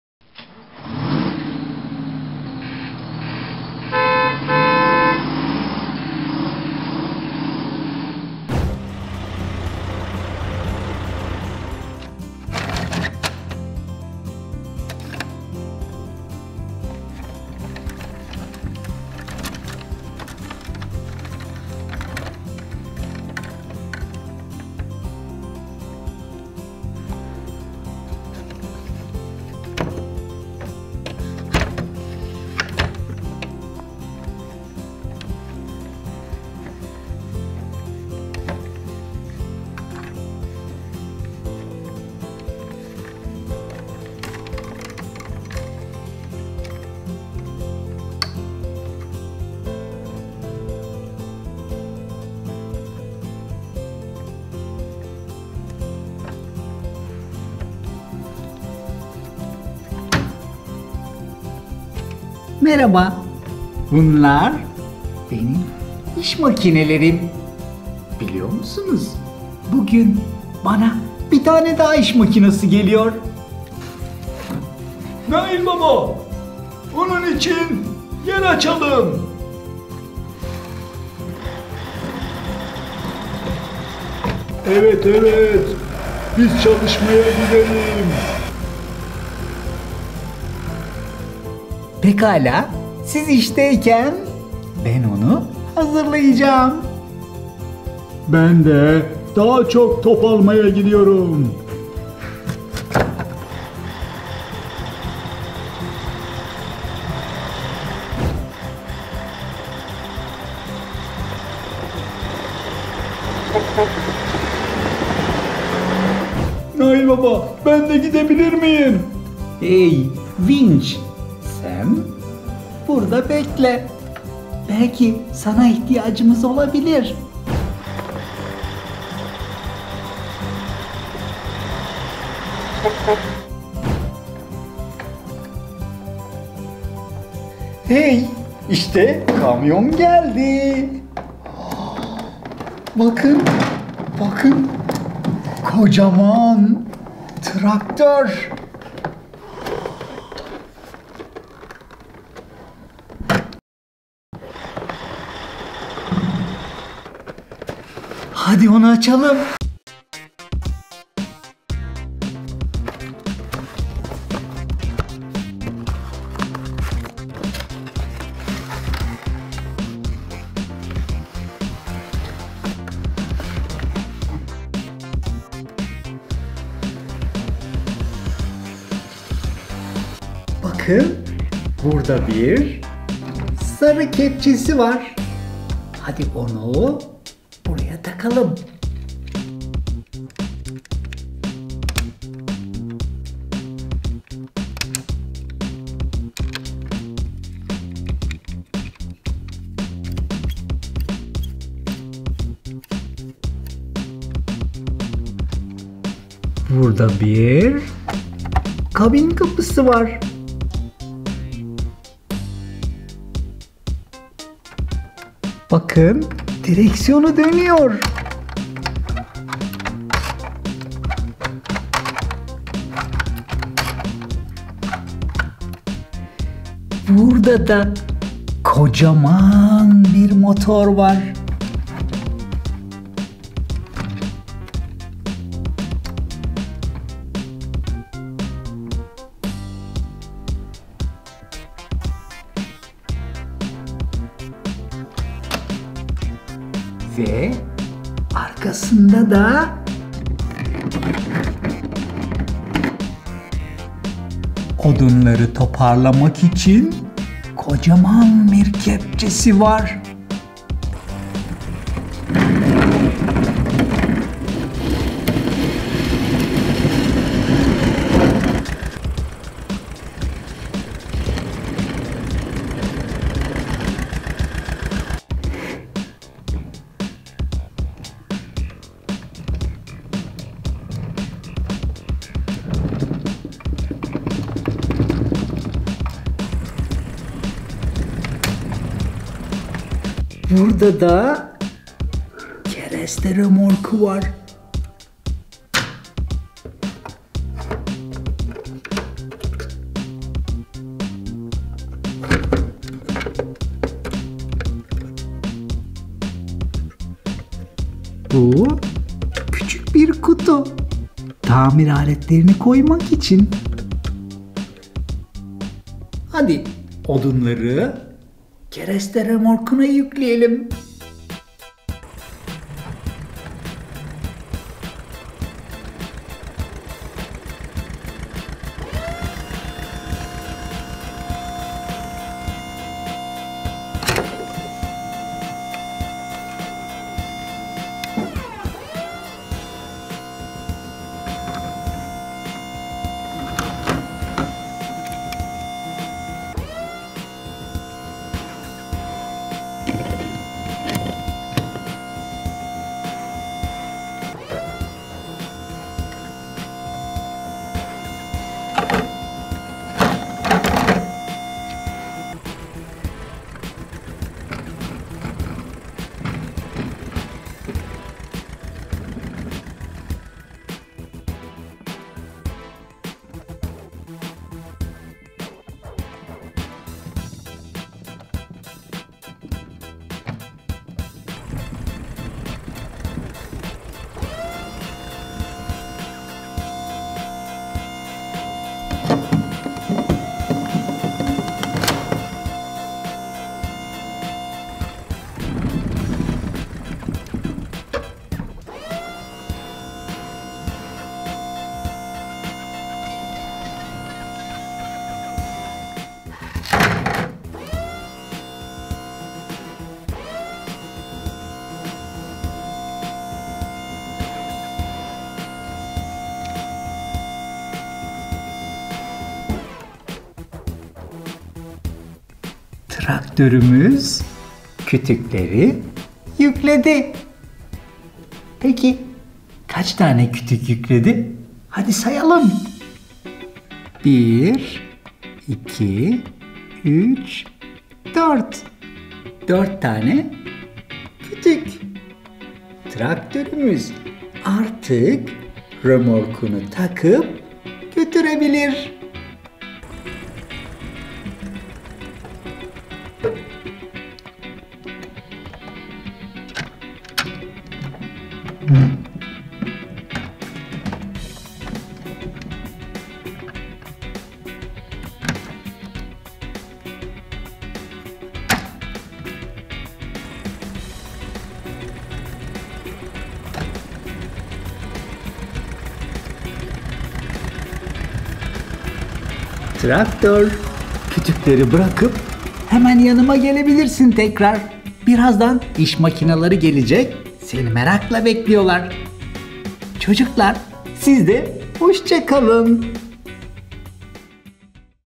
Traktör oyuncak. Nail Baba römorka kereste yüklüyor
Traktör oyuncak ile oynuyoruz. Bugün yeni oyuncak açılımı ve tanıtımı var. Traktör ve römork geliyorlar. Hadi açalım ve bakalım kutudan ne çıkar. Harika. Yepyeni motor, direksiyon dönüyor, kerestesi bile var. Hadi kepçe ile kerestemizi römorkuna yükleyelim.
Nail Baba ile çocuk oyunlarını izle